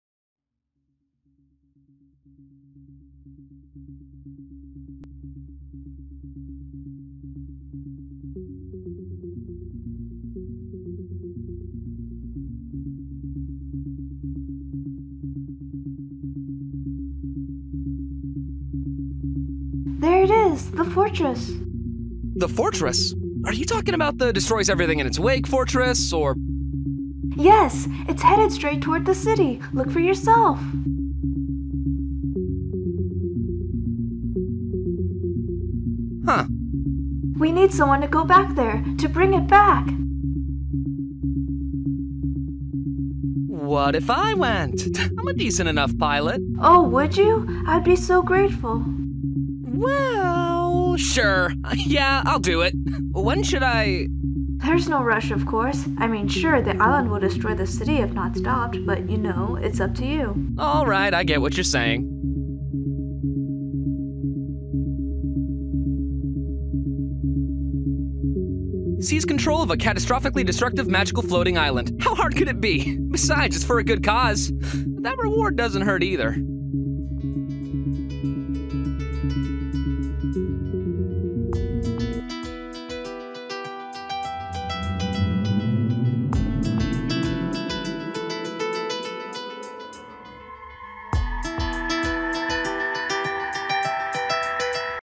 Sky Fortress OCT - Candy Beam Audition
This took... much, much longer than anticipated. But I also learned a lot about making a longer video! There's a lot I'm not happy with, but I know I'll be able to do better next time!

Big thanks to Frederator for letting me be part of their network!